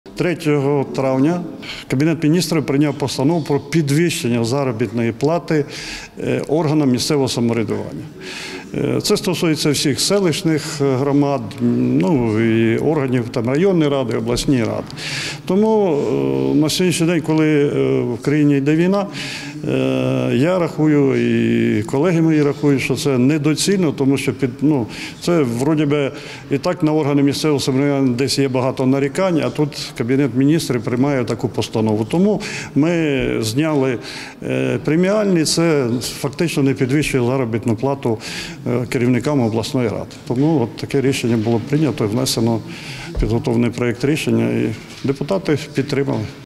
Депутати Житомирської облради проголосували за скасування премій для керівництва
У травні цього року Кабмін прийняв постанову про підвищення заробітної плати для працівників органів місцевого самоврядування та у Житомирській облраді відмовились від цього. Що вирішили депутати та чим аргументують своє рішення дивіться у відео